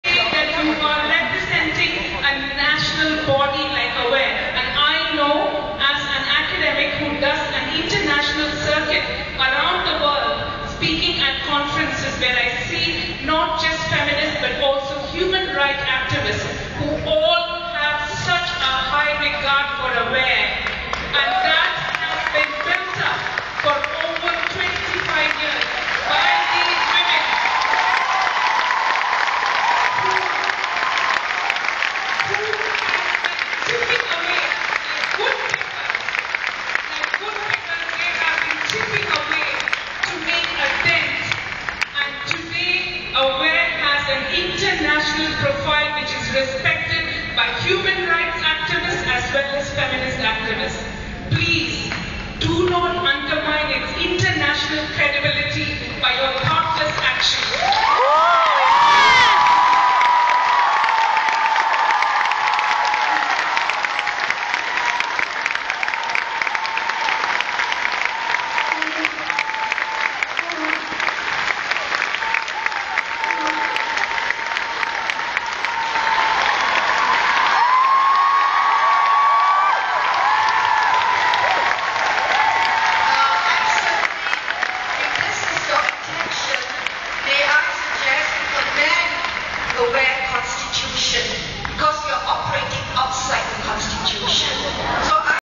Researcher_Concerns
A researcher voiced her concerns that Exco led by Josie will undermine the credibility of AWARE "as an international credibility by their thoughtless actions". Crowd applaused unanimously.